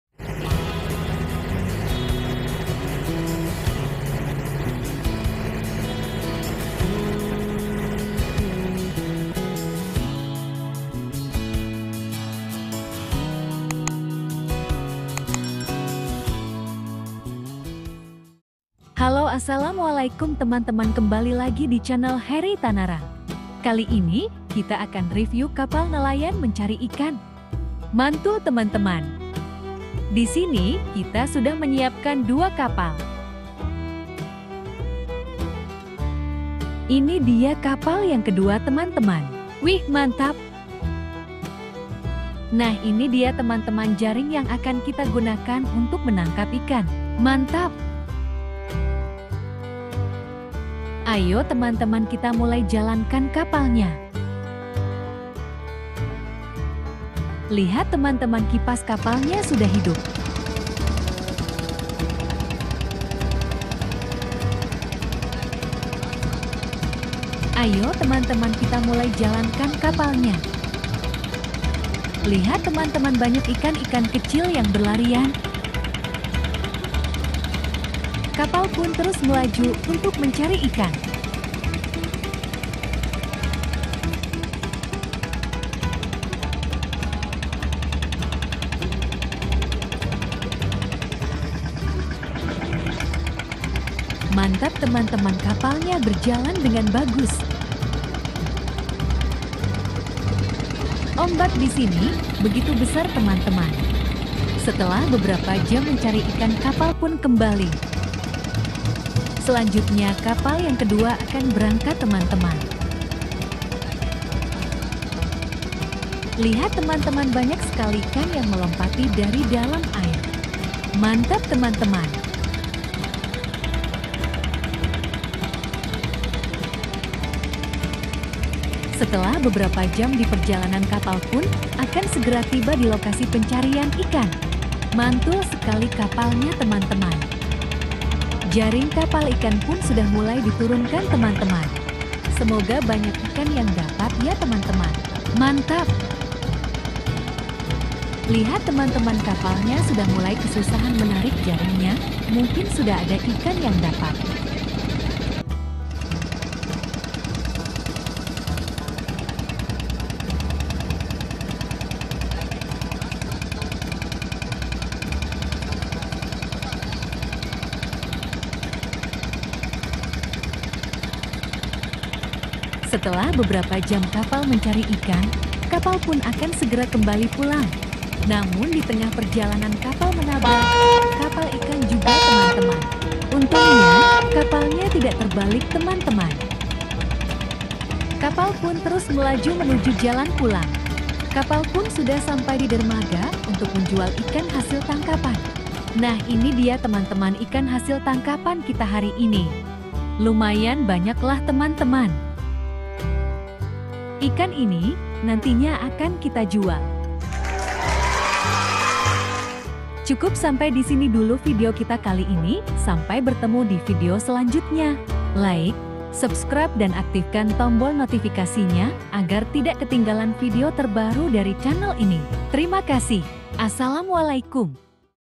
RC MINIATUR KAPAL IKAN MAINAN ANAK-ANAK‼️MENCARI IKAN, KAPAL MENANGKAP IKAN, KAPAL IKAN, KAPAL LAUT
Rc miniatur kapal mainan anak-anak, kapal mainan anak-anak bisa jalan sendiri
Jika teman-teman suka silahkan like, komentar, semoga menghibur.
jangan lupa sholat ya...